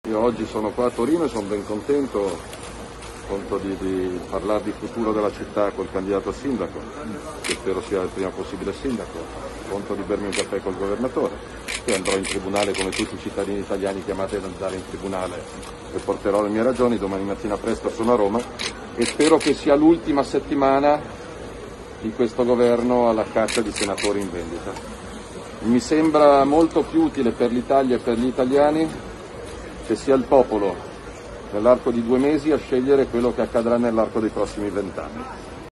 Dimissioni di Conte, Salvini: "Ultima settimana di un Governo a caccia di senatori in vendita"
(Agenzia Vista) - Torino, 25 Gennaio 2021 - "Spero sia l'ultima settimana di un Governo a caccia di senatori in vendita". Così Matteo Salvini, leader della Lega, a margine di un incontro a Torino. Facebook Salvini